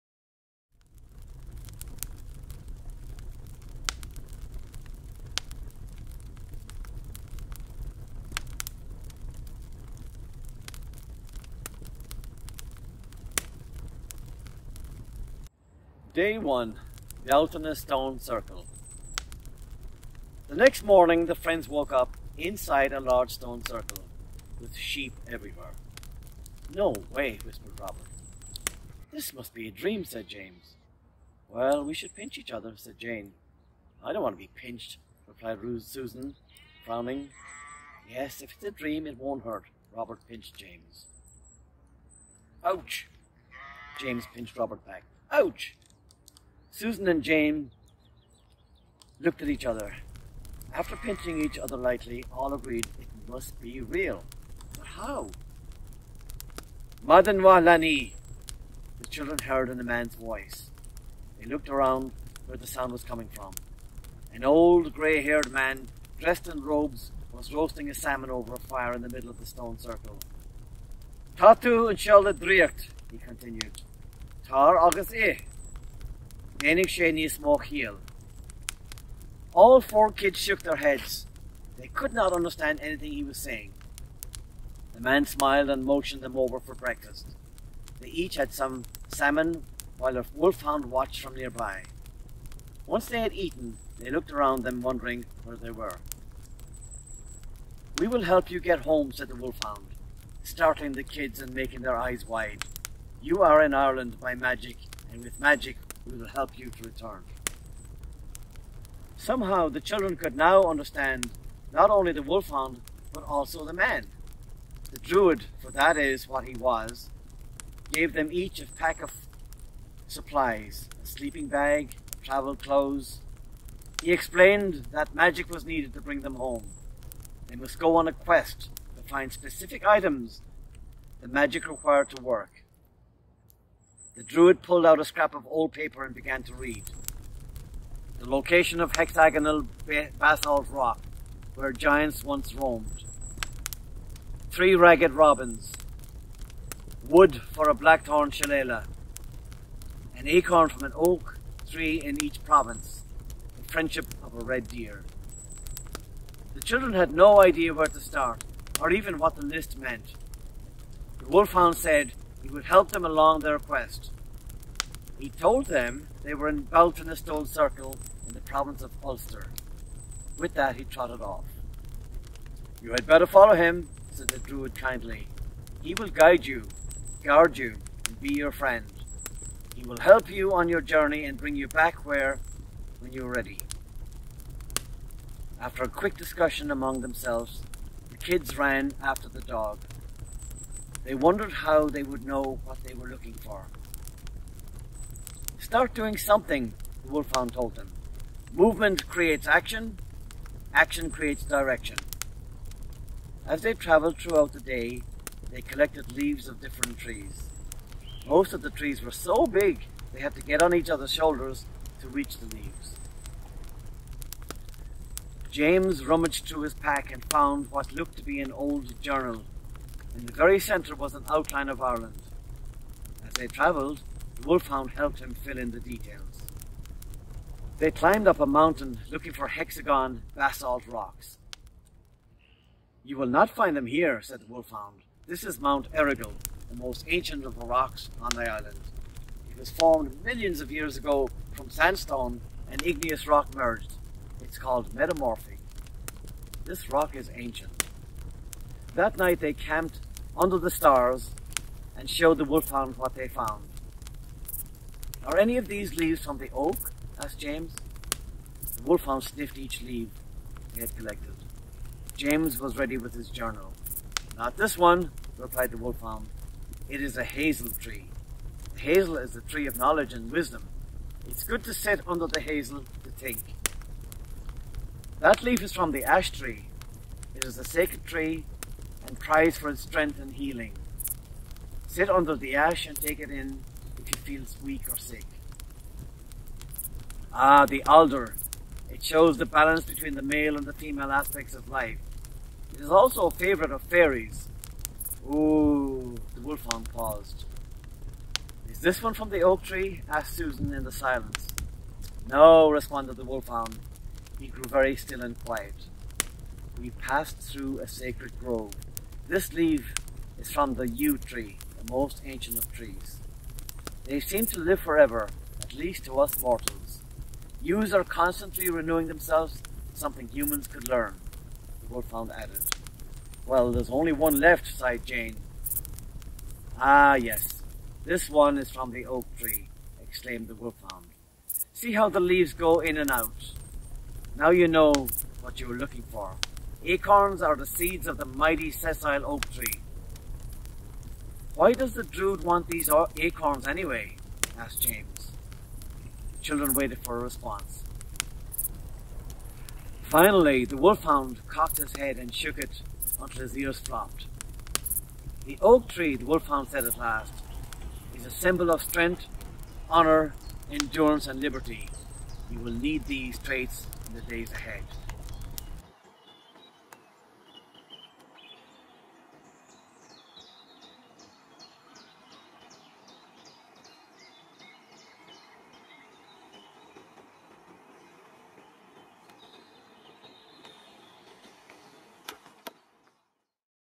Day One - Beltany Stone Circle: Through Time & Magic, An Irish Adventure
This Episode covers day one.  The children  wake up to find themselves transported to Ireland, meet a druid, their Wolfhound guide, and learn how to speak "animal".

Kieran O'Mahony reads the book Through Time & Magic: In Irish Adventure.  Along with the oration, video is interspersed to see the story come alive.

Four children wake up one morning to find themselves back in time and in another country:  Ireland.  In order for the magic to transport them home, they must embark on an epic journey.  With the help of the Wolfhound and Irish faeries, the four friends discover the magic of Ireland and within themselves.

This book was written for children to learn about the flora, fauna, geography, and geology of Ireland in a fun way.  To read the book, go to CelticLifeandHeritage.org